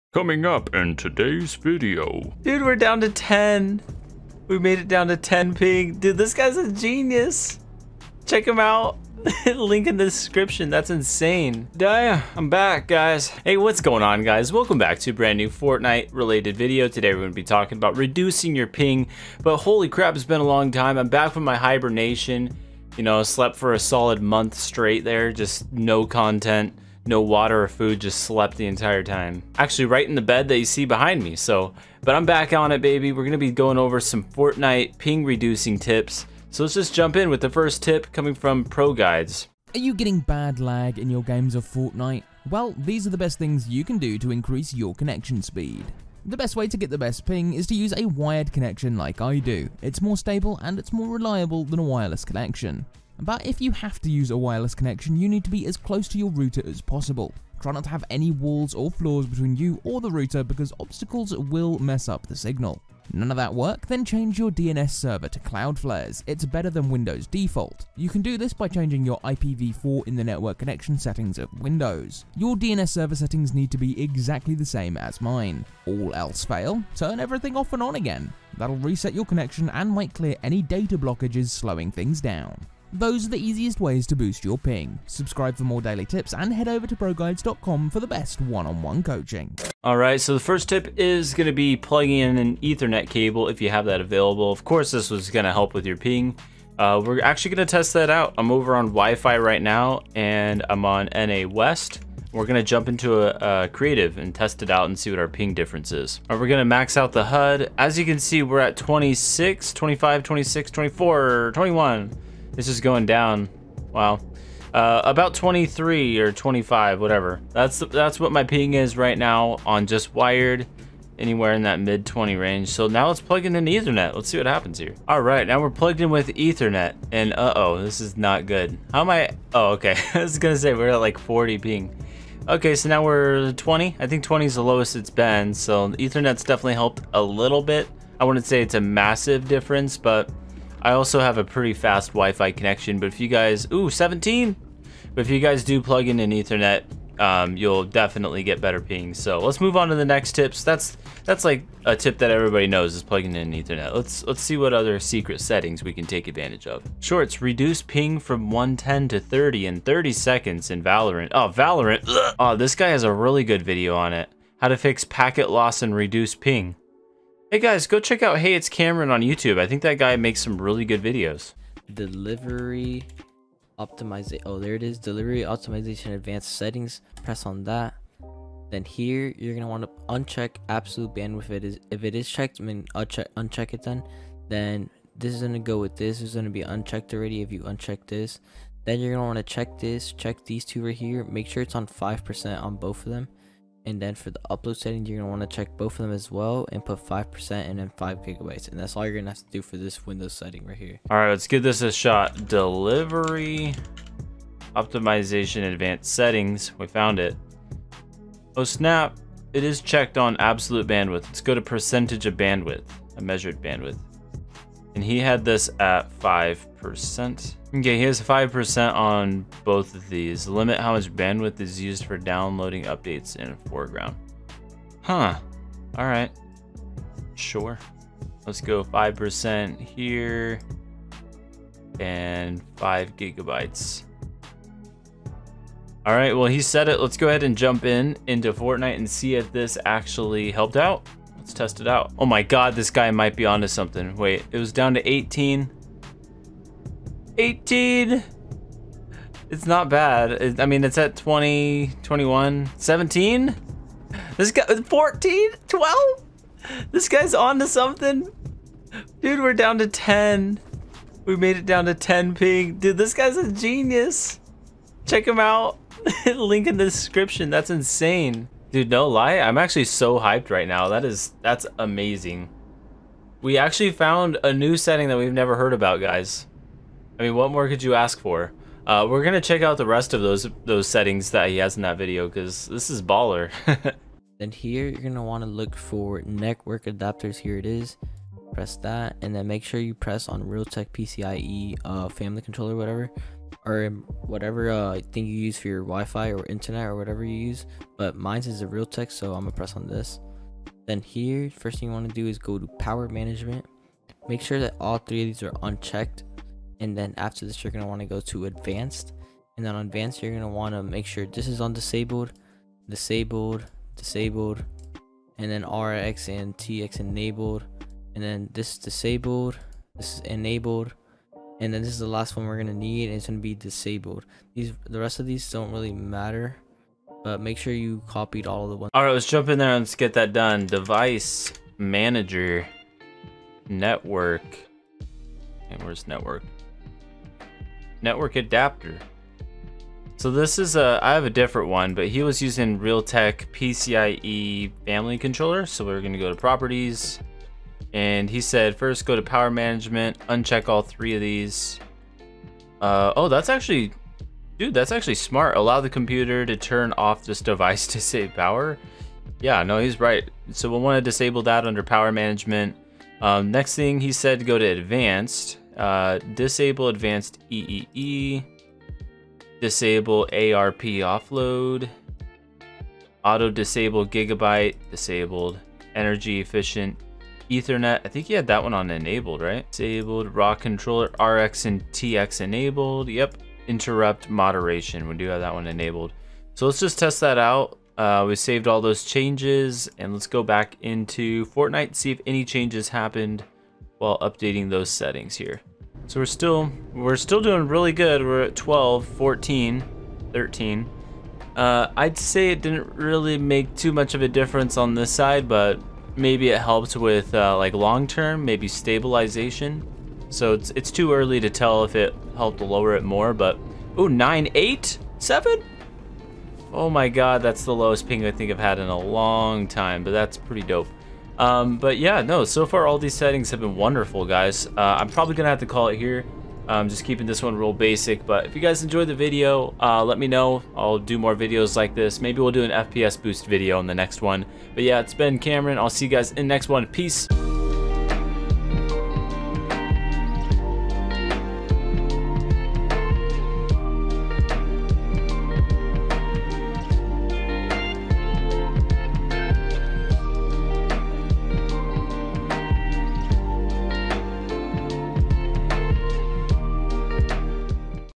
*HIDDEN* Windows 10 Settings GREATLY Reduce Ping & Fix Lag in Fortnite!
How to reduce ping and fix lag in fortnite, new settings that aren't talked about!